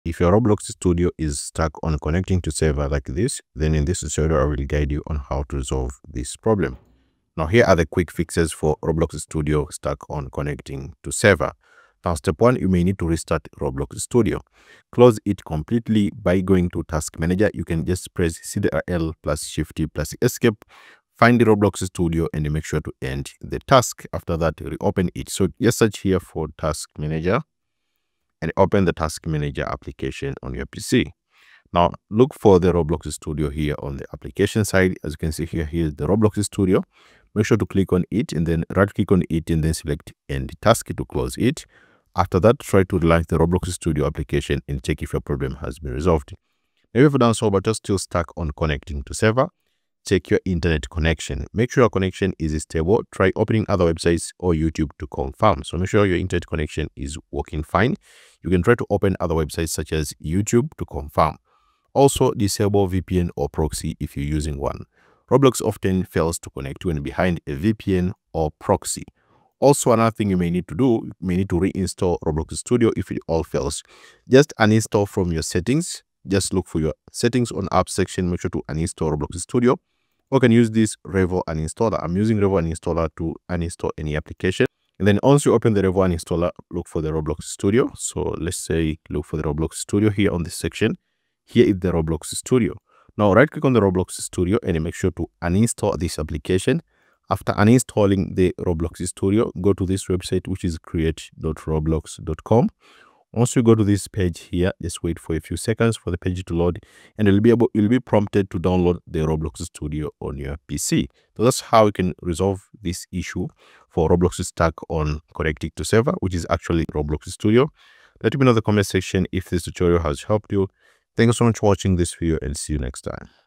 Roblox Studio stuck on connecting to server (Quick fix)
This tutorial will guide you on how to fix Roblox Studio stuck on connecting to server. Follow the steps below to resolve this issue.

✅ Quick Fixes

Restart Roblox Studio
Close it completely using Task Manager (Ctrl + Shift + Esc → find Roblox Studio → End Task), then reopen.

Make sure your connection is stable. Try opening other websites or YouTube to confirm.

Disable VPN/Proxy (if using one)
Roblox often fails to connect when behind a VPN or proxy.

Reinstall Roblox Studio
Uninstall from Settings → Apps.